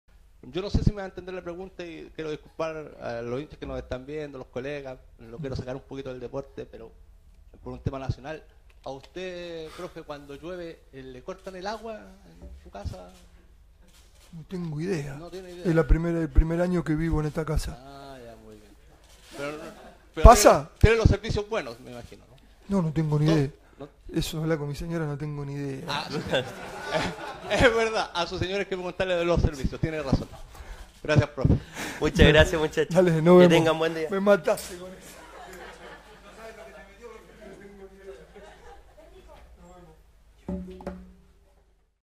Pablo Guede y el agua
Ante las lluvias de las últimas horas, un reportero le preguntó al DT de Colo Colo si sufrió algún corte del suministro de agua potable, como ya ha ocurrido en ocasiones anteriores... Algo que no entendió el estratega